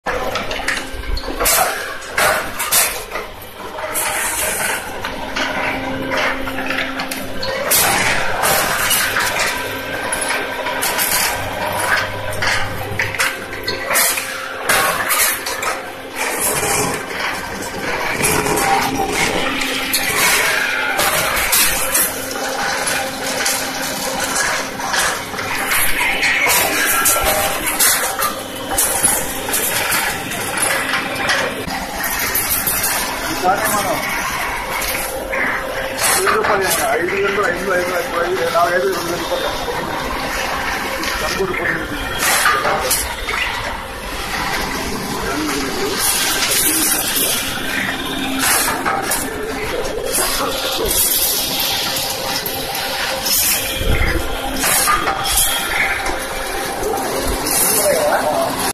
CEL;9014400331,Fully Automated Paper plates making machine (Wrinkle Dona Tiffin Raw material price)
For
PRODDATUR
KADAPA DIST
ANDHRA PRADESH
INDIA